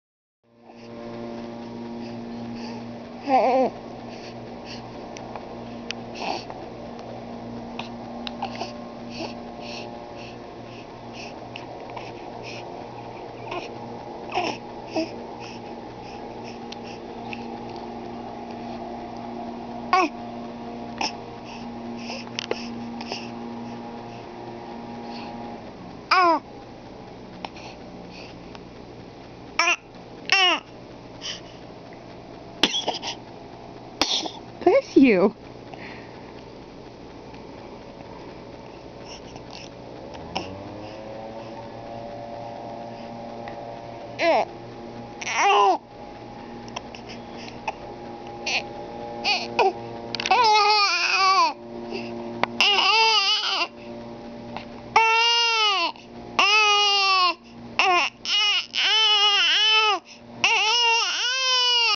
sneezy
matthew sneezes while playing airplane